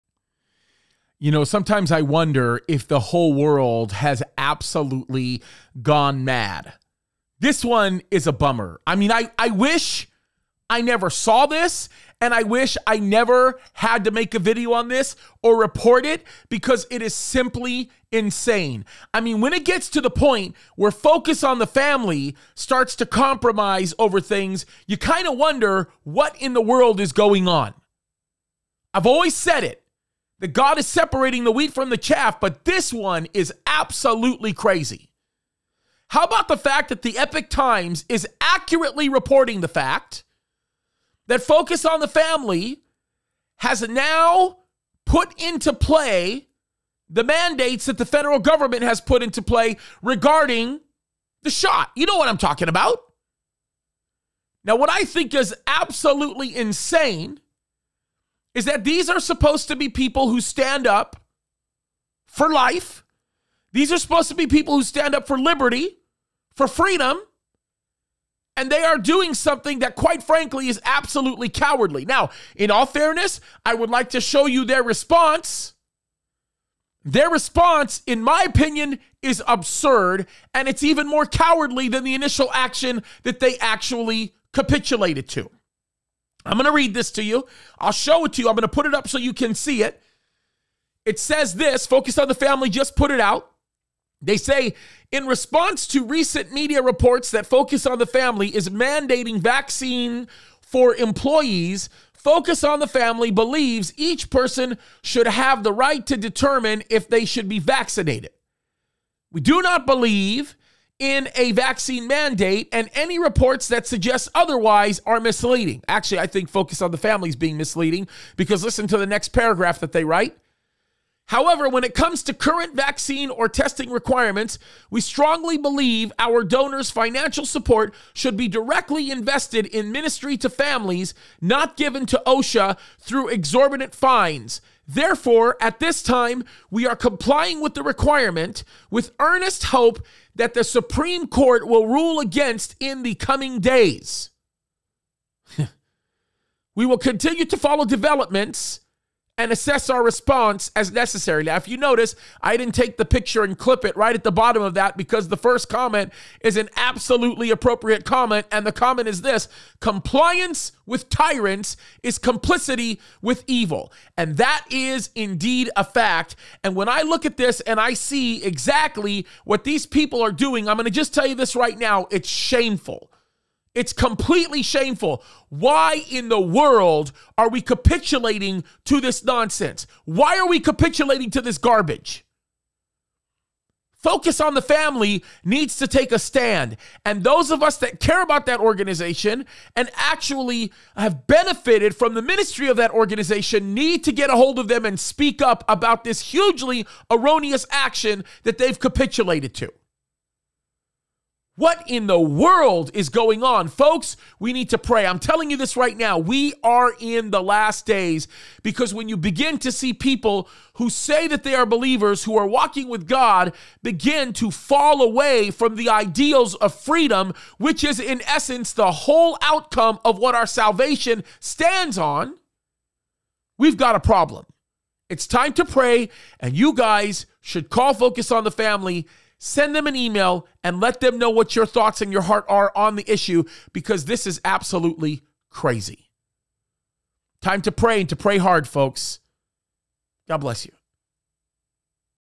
PLEASE TELL ME THIS ISN’T REAL!!!
I can’t believe what I’m seeing and I’m overwhelmed with sadness. Complicity with tyranny is fellowship with darkness and focus on the family needs to think twice about what they’re doing!!!